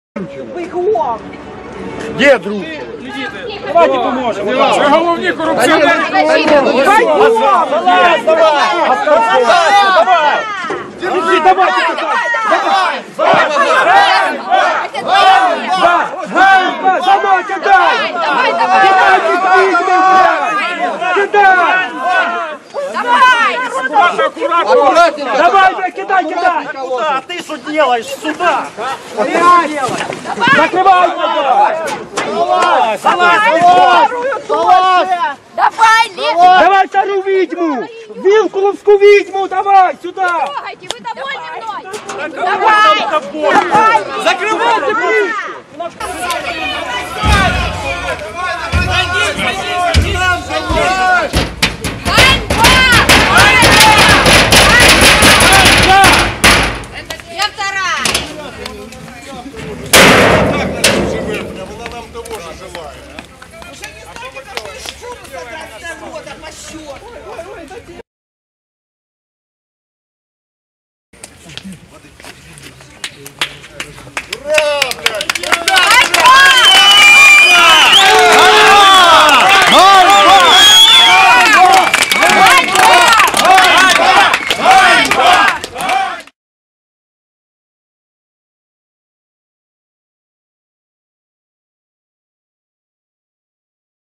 Euromaidan - Corrupt Ukrainian official is thrown into the garbage can
With credits to Chistaya-pravda
The present material selected, adapted & published by EMPR #1 independent citizen media about Ukraine | A Corrupt Ukrainian Official Is Thrown In The Garbage Can, Nov 1 2014